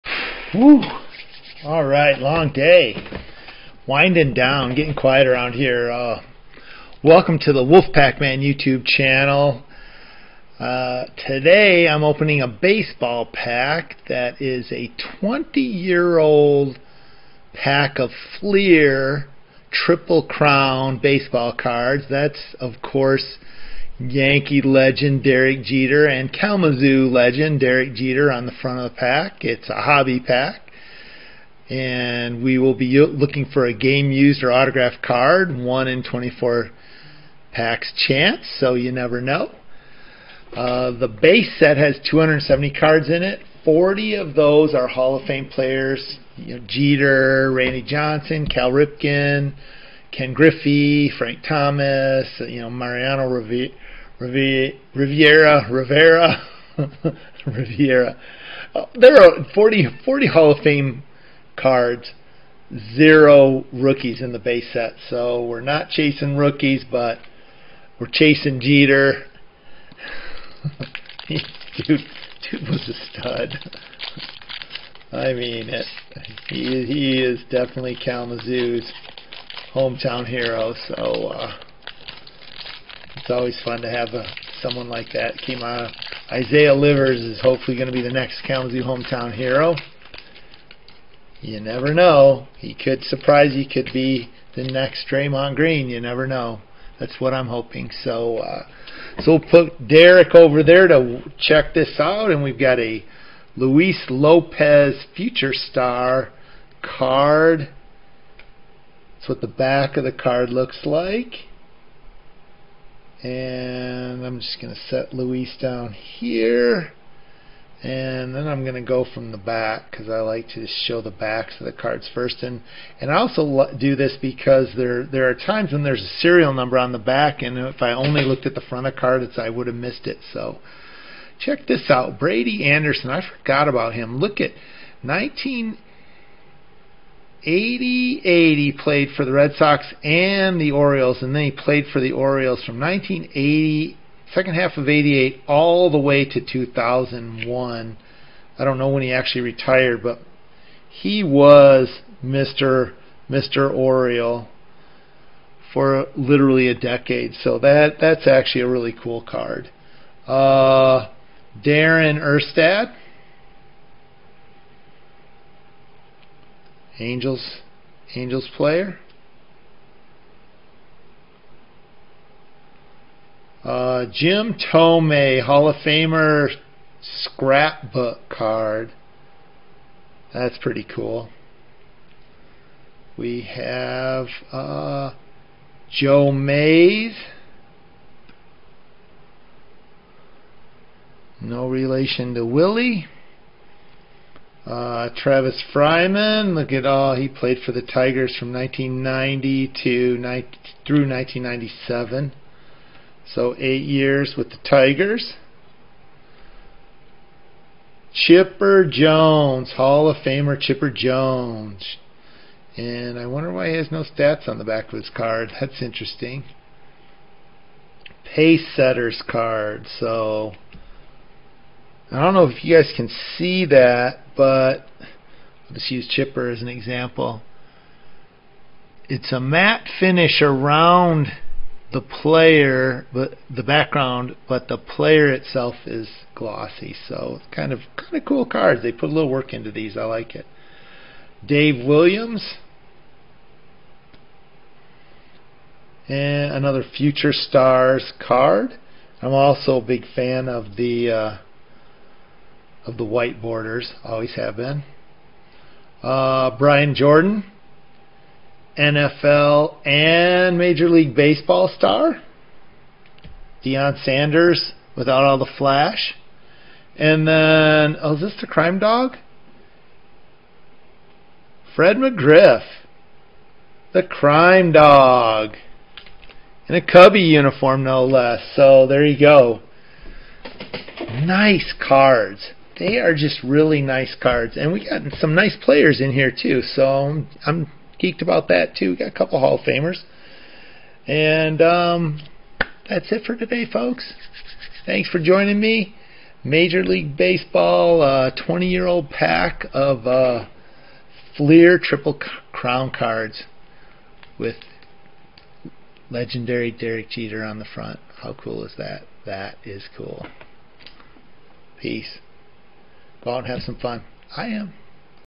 2002 FLEER TRIPLE CROWN HOBBY DEREK JETER baseball pack w/Chipper Jones Jim Thome & Fred McGriff!
One pack of 2002 Fleer Triple Crown baseball cards.

This hobby pack has 10 cards and Yankee legend Derek Jeter on the front!

The base set has 270 cards
40 being hall of fame players like
Derek Jeter,
Randy Johnson,
Cal Ripken Jr.,
Frank Thomas,
Ken Griffey Jr.!

There are 0 rookies in the base set.

Found: Chipper Jones
Jim Thome,
Fred McGriff,
Brian Jordan!